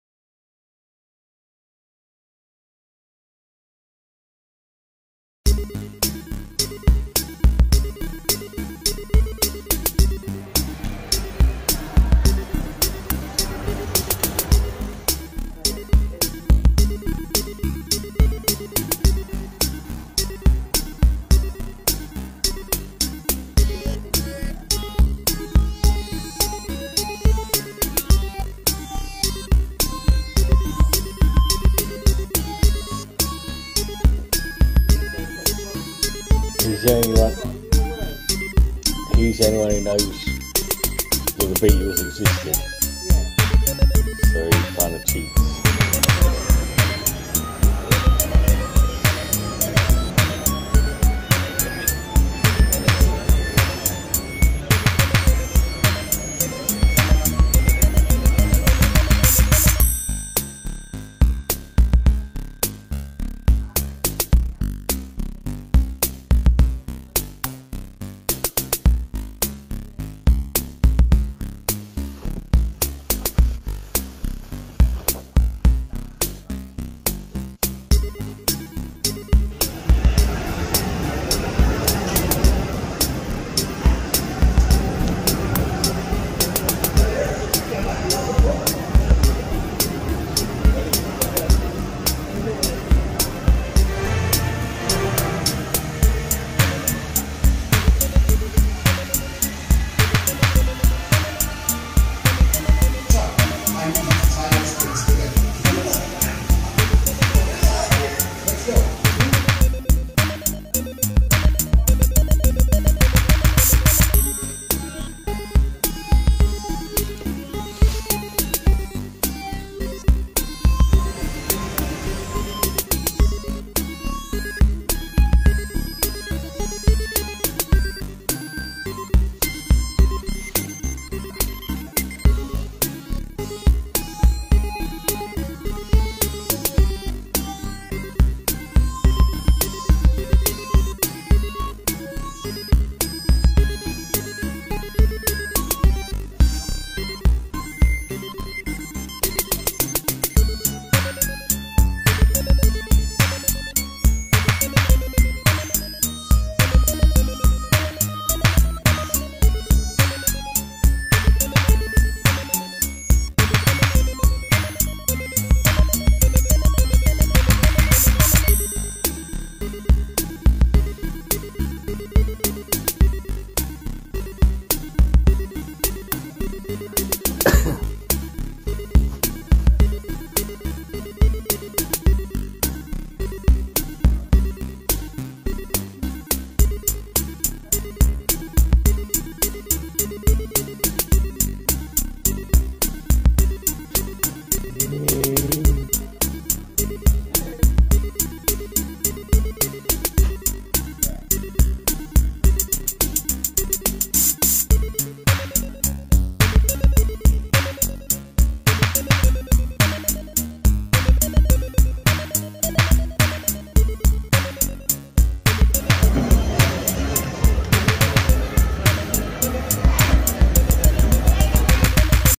Angry Birds World Theme Park Qatar, Doha Festival City, An Angry Birds Land in the Middle East 2023
Angry Birds World Qatar, An all weather theme park at Doha Festival Mall based on the hugely popular Angry Birds franchise with some well themed rides, a cracking go kart track and a superb centrepiece Slingshot based super slingshot